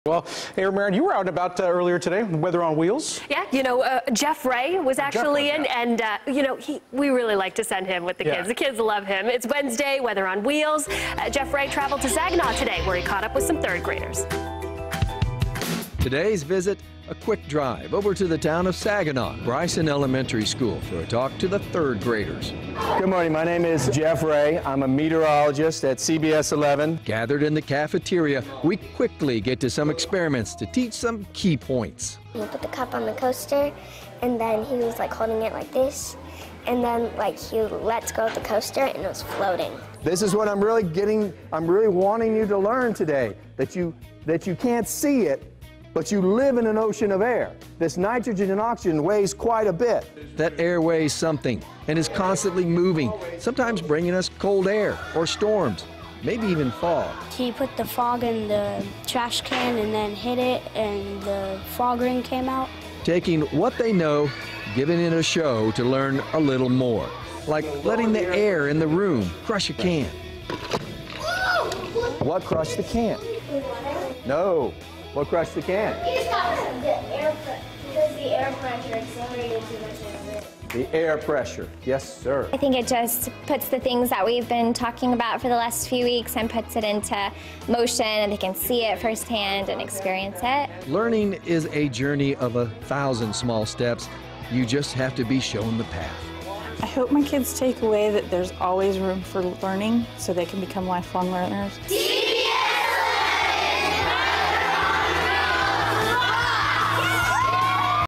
Bryson Elementary School
Our CBS 11 meteorologists headed out to talk about weather, and some fun science stuff!